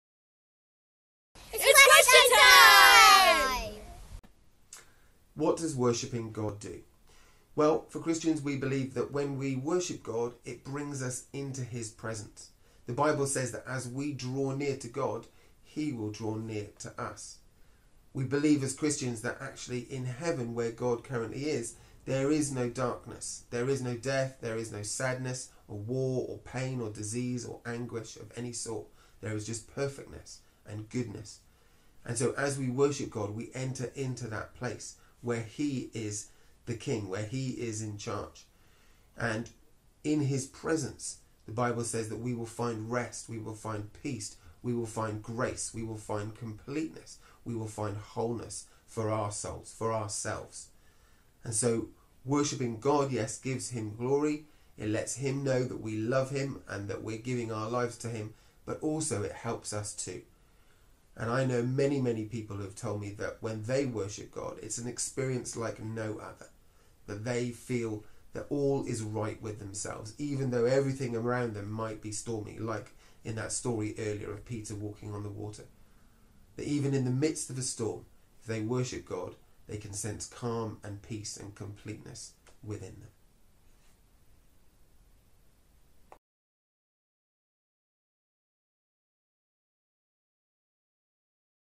What does worshipping God do?
Matt answers the question we frequently get asked in schools, "What does worshipping God do?"